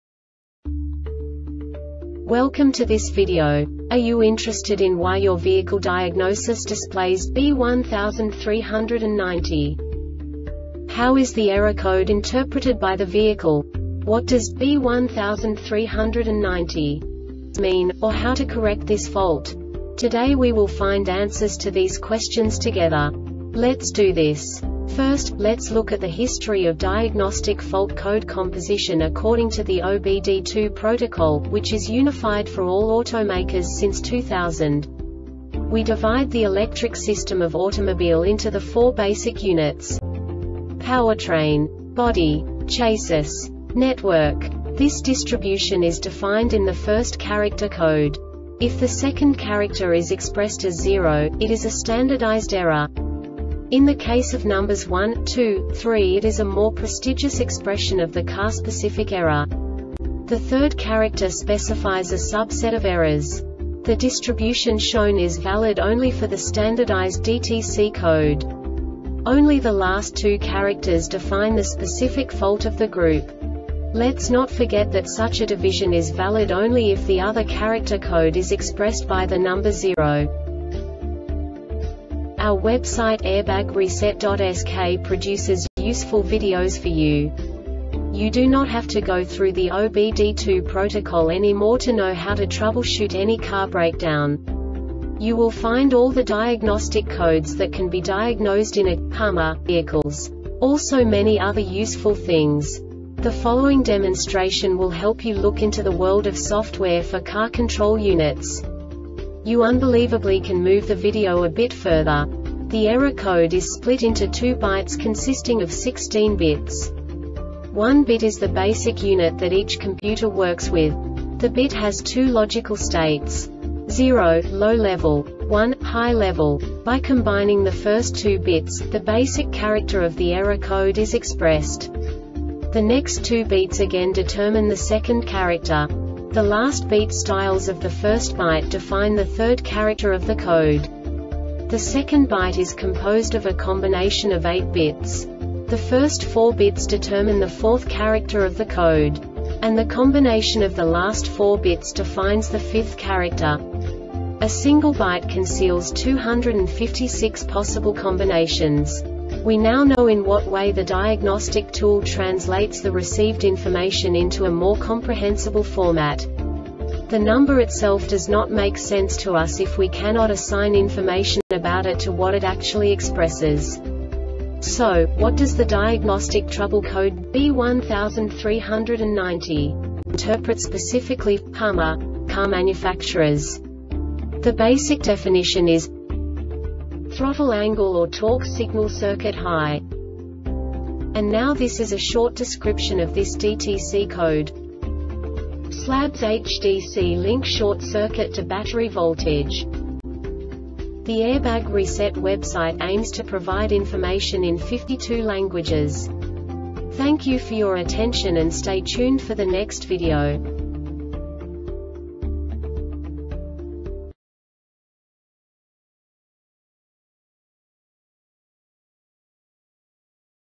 DTC Hummer B1390 second Short Explanation
Contents:
0:21 Basic DTC analysis according to OBD2 protocol standard.
1:48 Insight into programming
2:58 A diagnostically understandable description of the Diagnostic Trouble Code
3:05 Basic error definition

Make:
HUMMER
Code:
B1390
Definition:
Throttle angle or Torque signal circuit high
Description:
SLABS HDC link short circuit to battery voltage